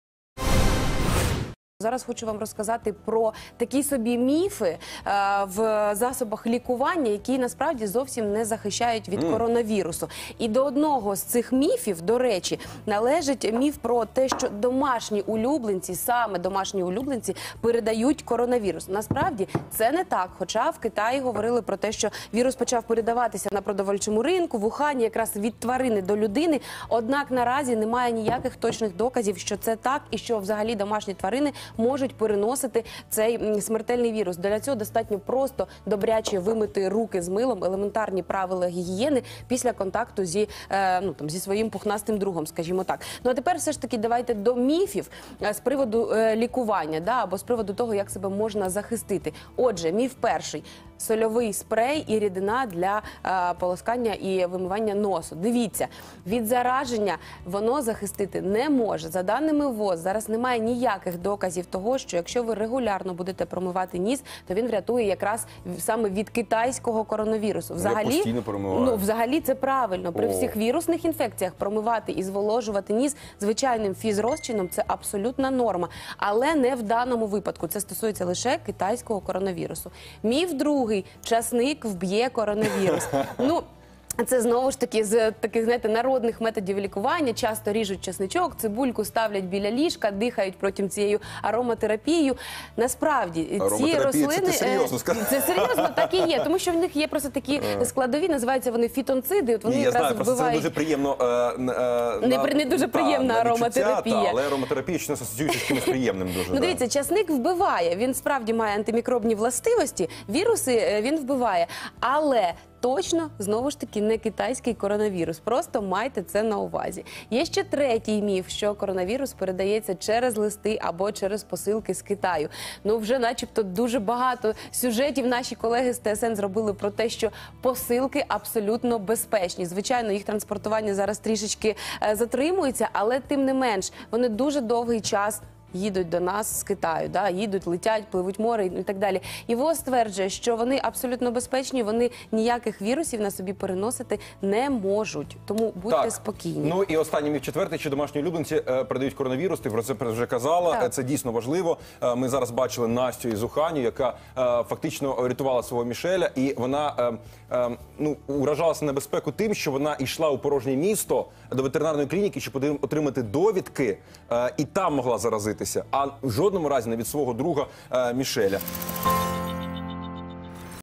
5 мифов про коронавирус
Нужно ли дезифецировать посылки из Китая и насколько опасны домашние животные смотрите в сюжете.